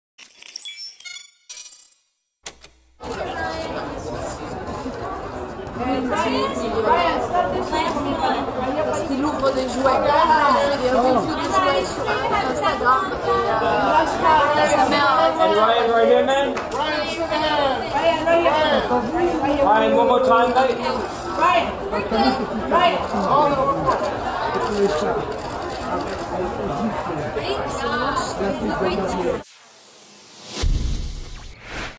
Ryan Kaji 2019 Kids' Choice Awards Orange Carpet
Broll footage: Ryan Kaji on the orange carpet at the Kids' Choice Awards held at the USC’s Galen Center in Los Angeles, California USA on March 23, 2019